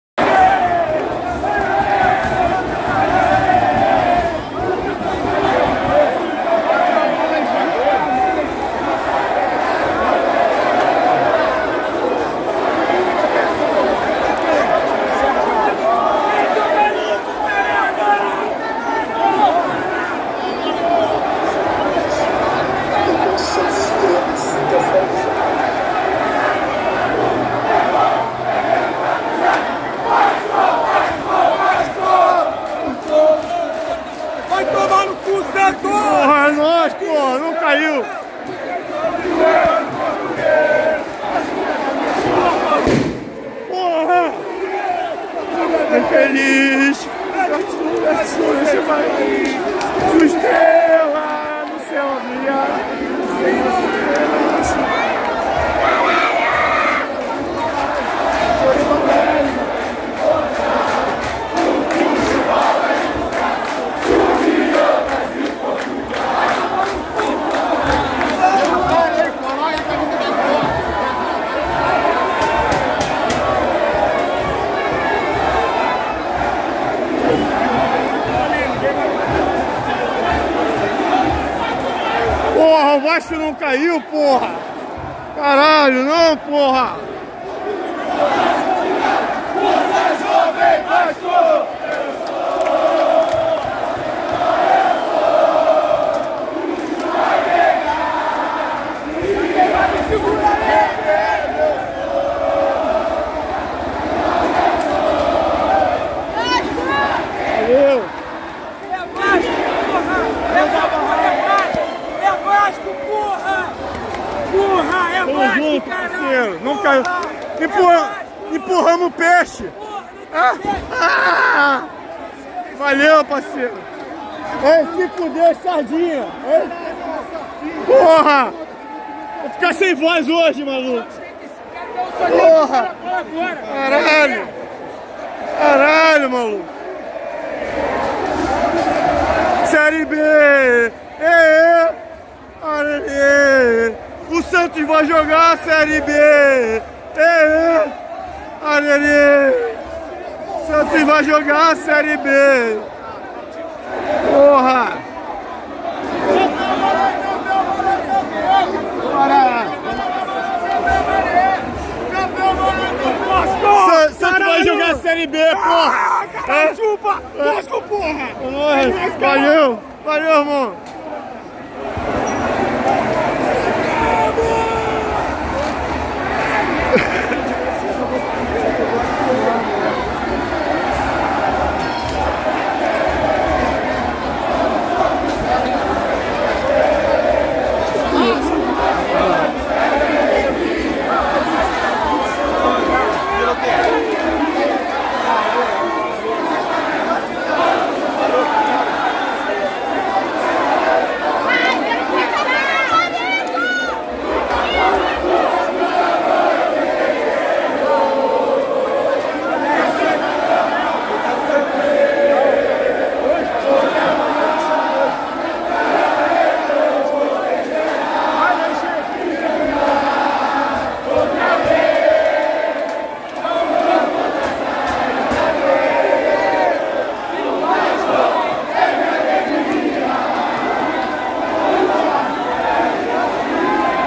VASCO 2 X 1 RB BRAGANTINO - OS BASTIDORES DA TORCIDA VASCAÍNA EM SÃO JANUÁRIO. - 06/12/23
Seja um membro por apenas R$ 4,99 por mês e ajude o canal a continuar cobrindo os bastidores da torcida nos estádios. 💢❤👇🏻